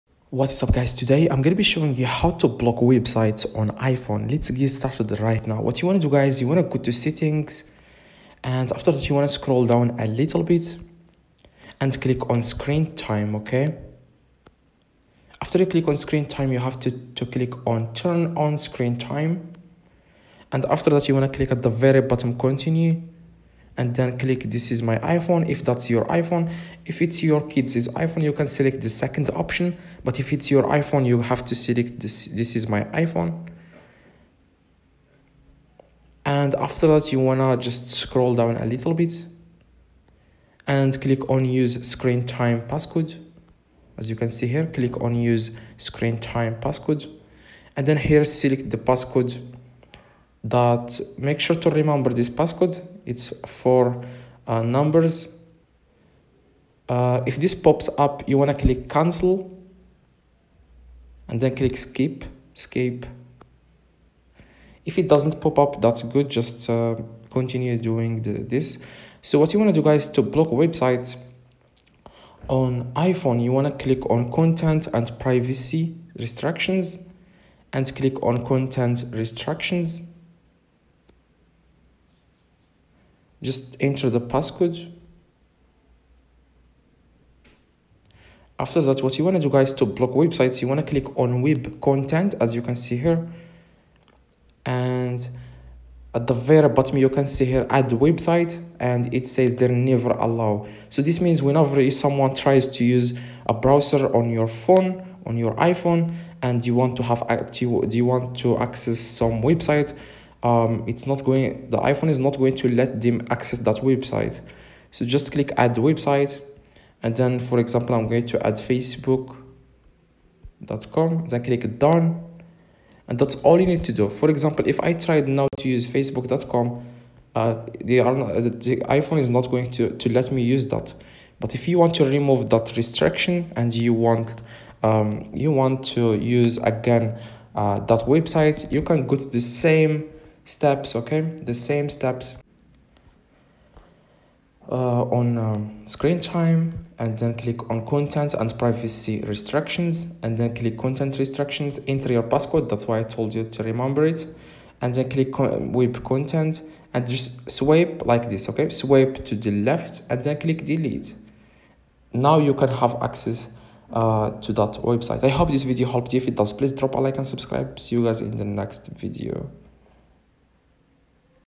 How to Block Websites On iPhone ( Easy Method) | Block Specific Sites on iPhone
In this video I'll be showing you how to block websites on iPhone after watching this you'll learn how to block specific sites on iPhone on safari or chrome (easy)
If you want to set up an iPhone and iPad for your child, you may consider restricting access to any websites that you'd rather not end up in front of young eyes.
How to block websites on an iPhone and iPad? follow the steps in the video.